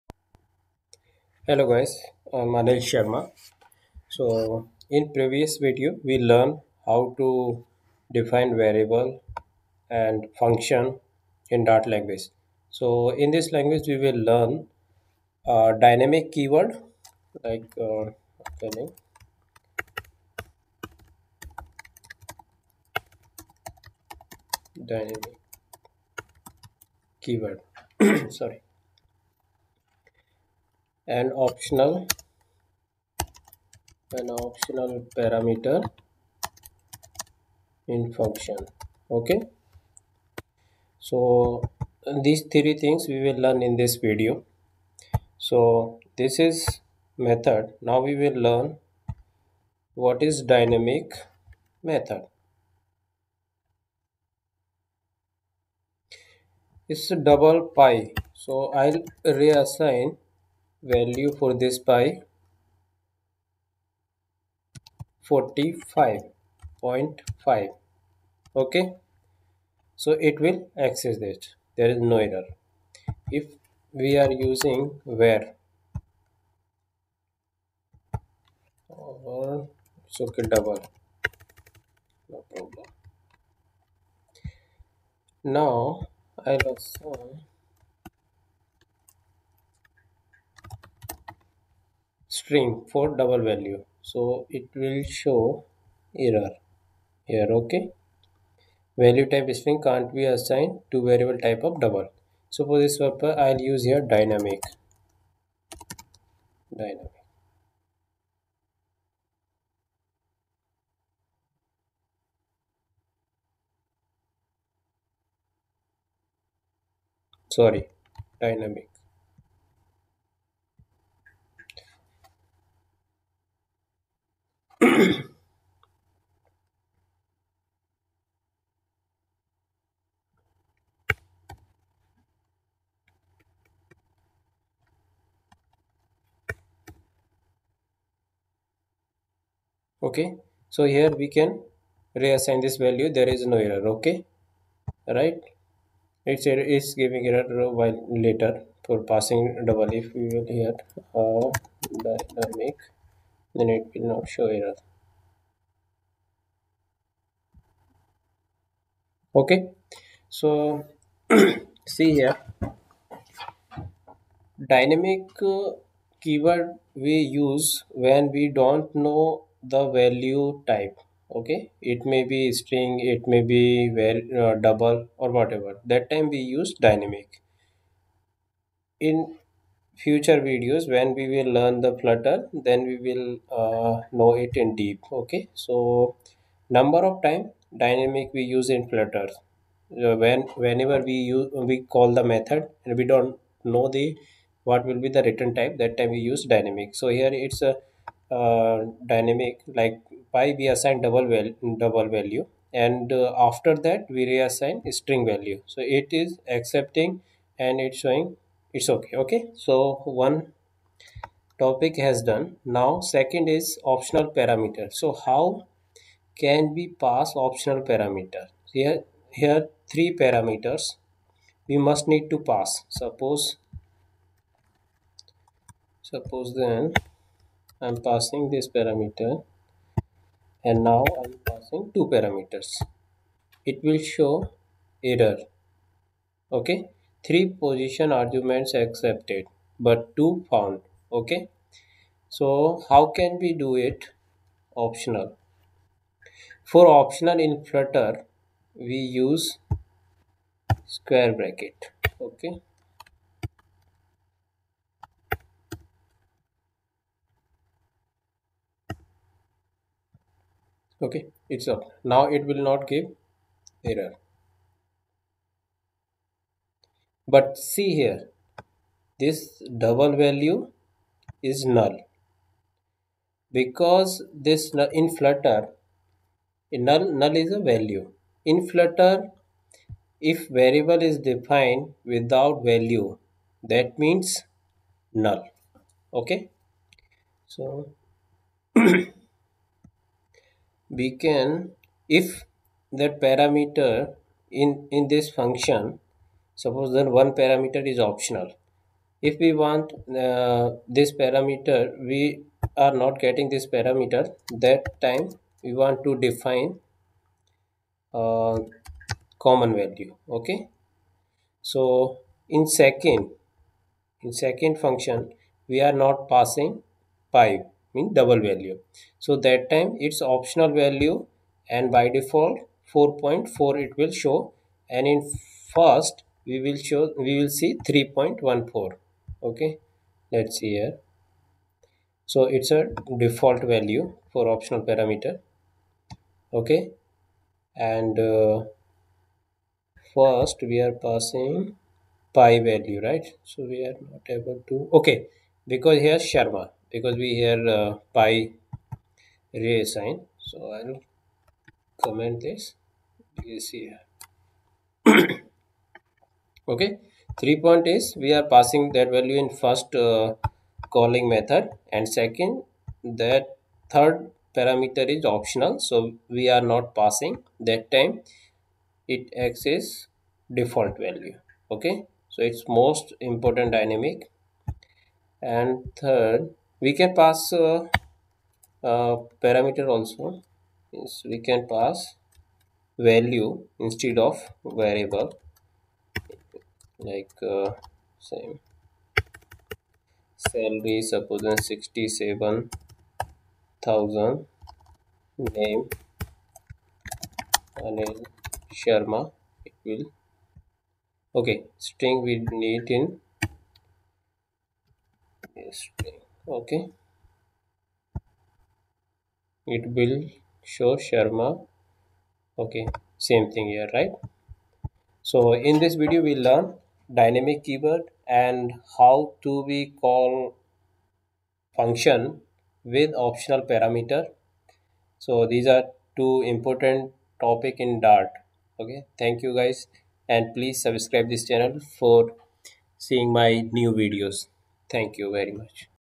Dart Language Part-2 | Dynamic keyword, Optional Parameter of function.
- Learn What is dynamic keyword in dart Flutter.
- An optional parameter in dart function.
- Optional field of function.